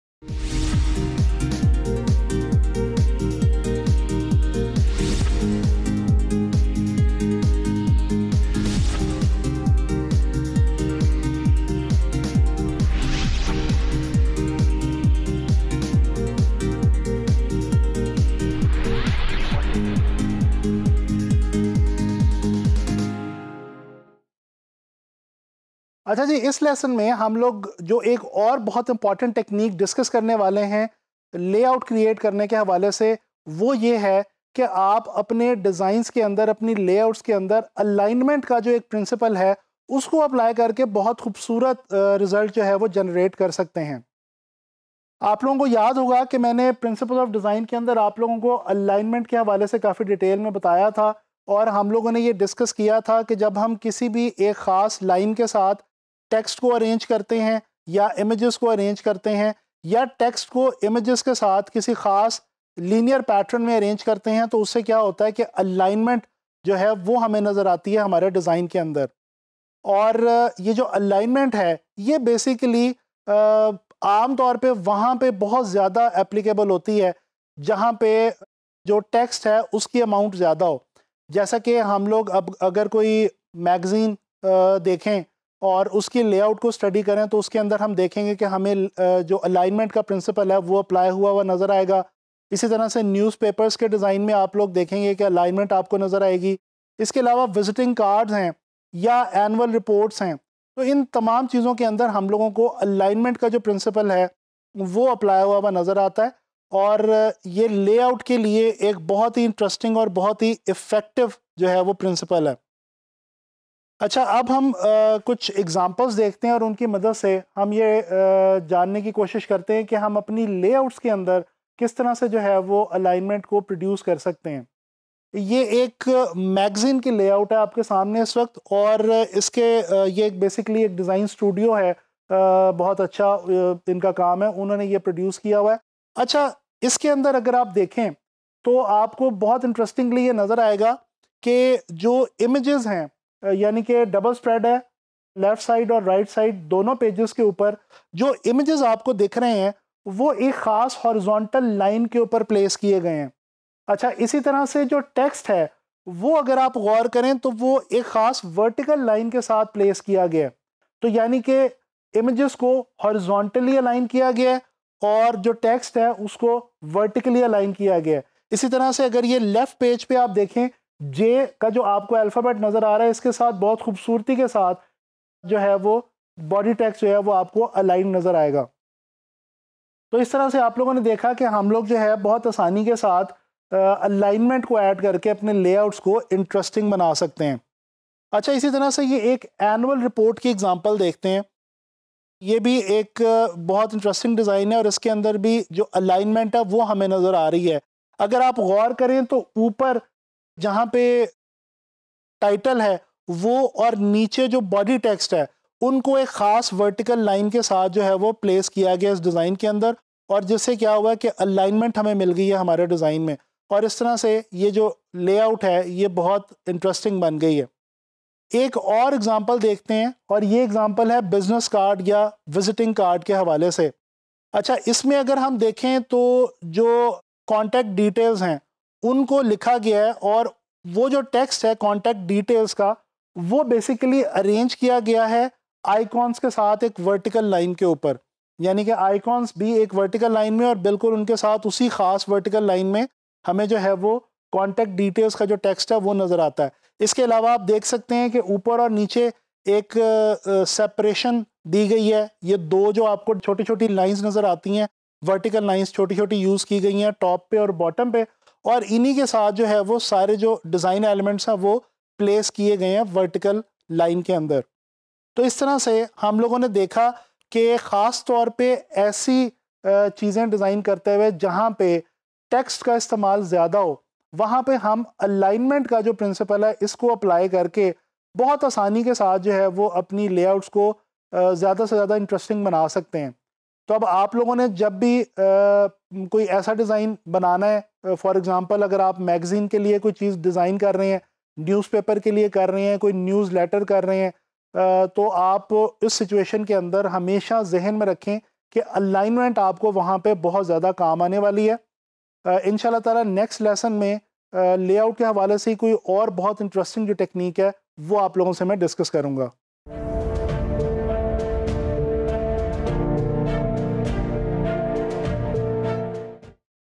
Topic 152 | How to Align Your Elements in Adobe InDesign | Graphic Design
Topic 152 | Theory Important Techniques to make interesting Layouts Compositions in Adobe InDesign, How to  Align Your Elements in Adobe InDesign | Graphic Design

In this topic, you will learn how to align different element(s) while designing a layout/composition. The use and benefits of this technique will also be shared with the help of an example.